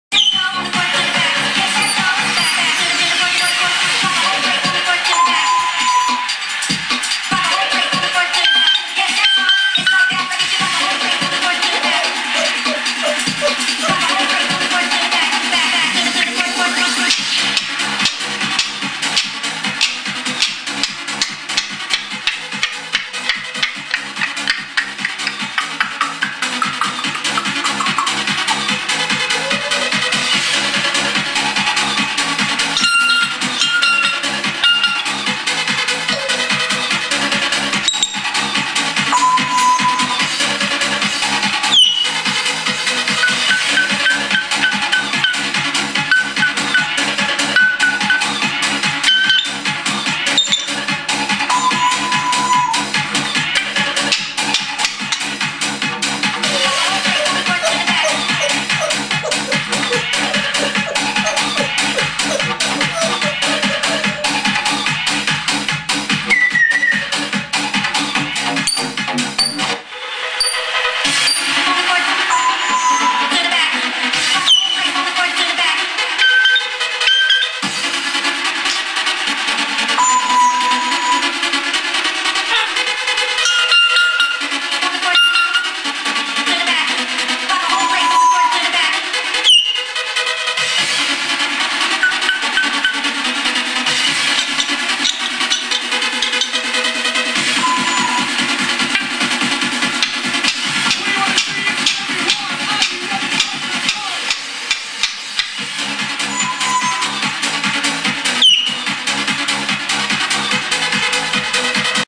MY AFRICAN GREY PARROT EMCEE..Billie-Girl Grey
BILLIE GIRl GREY  ENJOYS OLD SKOOL MUSIC.
DJs MIKE DAVIES & FUNKI B.
Billie Girl Grey really freaks to the music in this video.